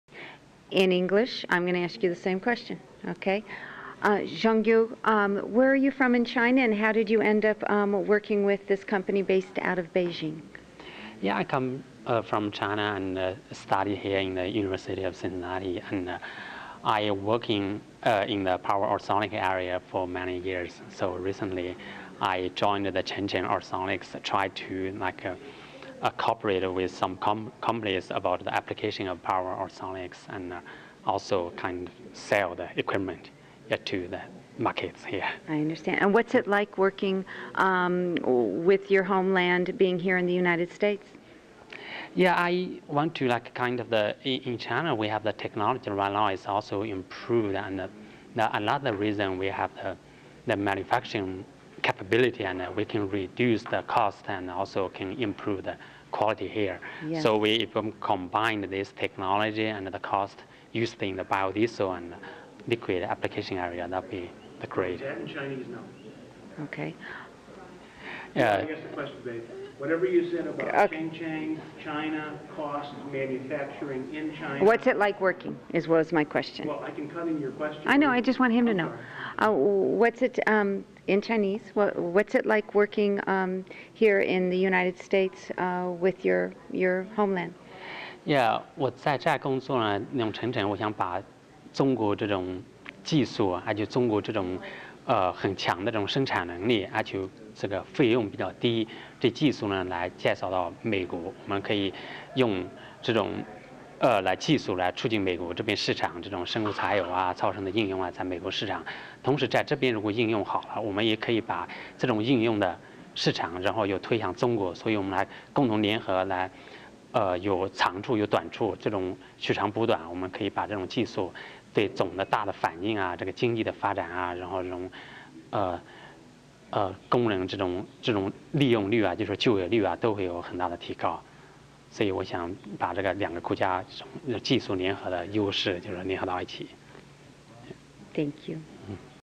QUENCH! BioDiesel UltraSonic Phyto-Tron & SLIME Lubricant
QUENCH! BioDiesel Equipment & Lubricating Oil Company
uses SonoChemistry - Ultrasonic, Microwave, Centrifuge
in THE PHYTO-TRON - A 1,000 Gallon Per Day Capacity
BioDiesel Reactor for under $10,000.00 after Govt. Rebate.
The "SLIME" lubricating oil is ASTM grade BioDiesel
produced by 10,000  independent cooperatives world-wide
producing 100,000 green BioDiesel Recycling Jobs.